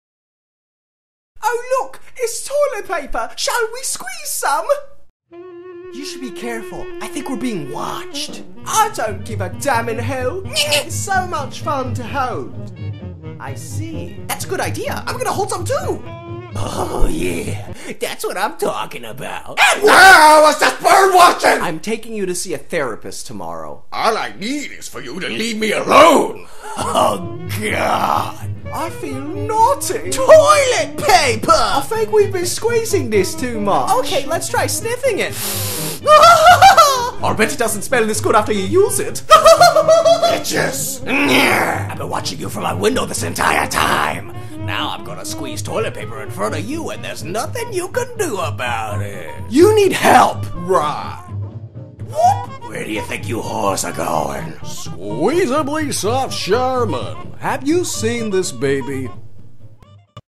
Crazy Ass Banned Toilet Paper Commercial
Can you guess why this one was banned? Here's a hint: Keep your eyes on the old man's hands while he's "birdwatching".

This Charmin commercial was one of the first to ever feature a missing baby on the package like a milk carton. The company's owner Donald "Charmin'" Charmin thought people would have a chance to look over the image while taking a dump and that would be the best possible time to mentally absorb the image of the child so that it could one day be recovered. It was a nice idea, but sadly it didn't work and now the Charmin baby is their official mascot. Minor facial characteristics were changed to keep it from being too insulting to the baby's family, butt in a way, his memory lives on forever.